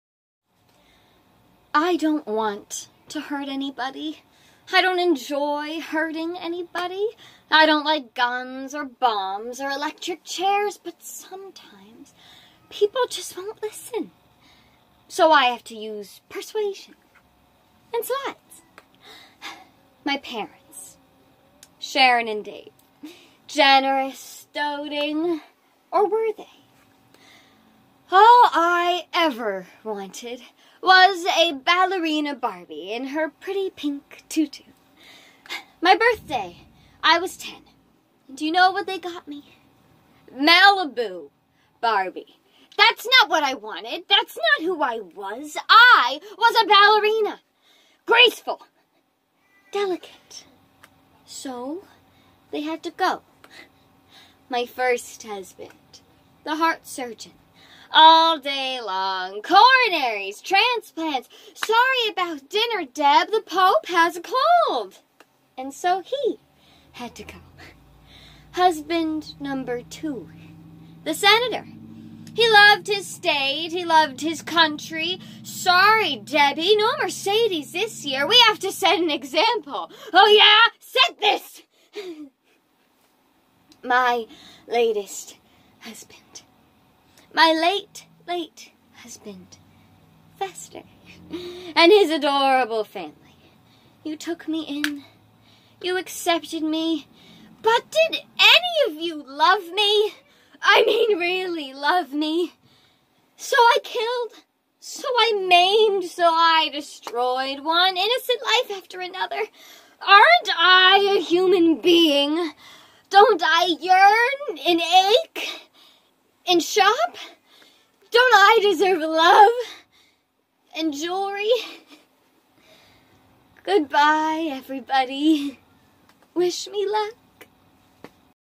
Debbie's Monologue - Addams Family Values
So I just had to do this for a performance class mini assignment and it wasn't even memorized but I had a lot of fun with it so here it is 😂